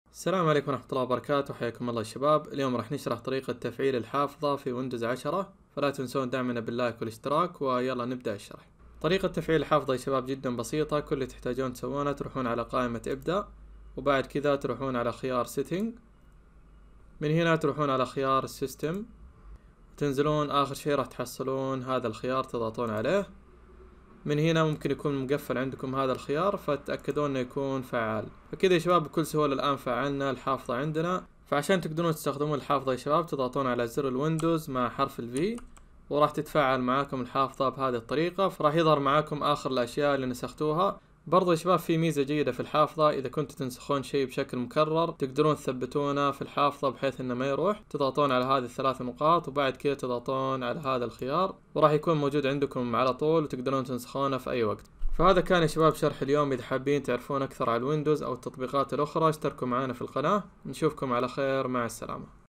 طريقة تفعيل الحافظة في ويندوز 10 واستخدامها في النسخ و اللصق
في هذا الفيديو رح نشرح طريقة تفعيل الحافظة في ويندوز 10 واستخدامها في النسخ و اللصق المتعدد بكل سهوله خطوة بخطوة وفائدة الحافظة في انه يمكنك استرجاع النصوص الي نسختها سابقا بكل سهوله